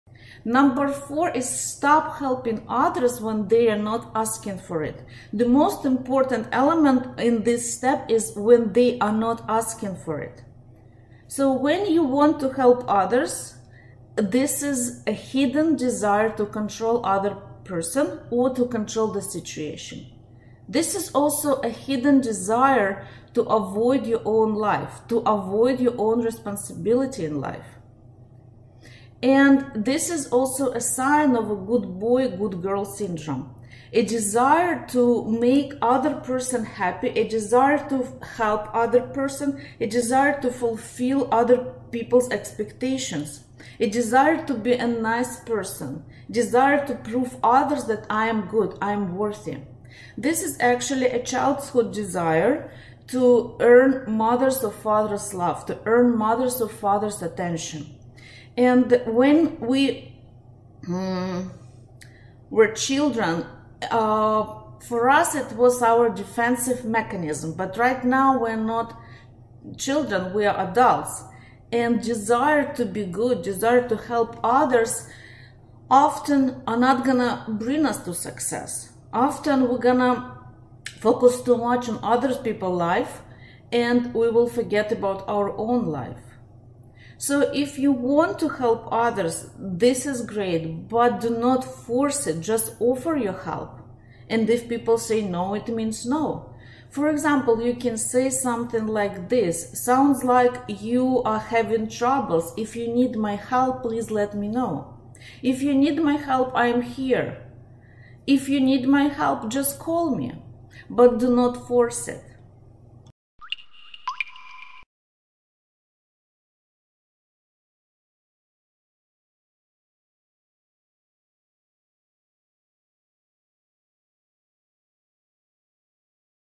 Stop being nice. Stop people pleasing. People pleaser syndrome. Life coaching tips by Elena Semenek
Elena Semenek
The founder of Psychology of Happiness, where happiness is the purpose of life!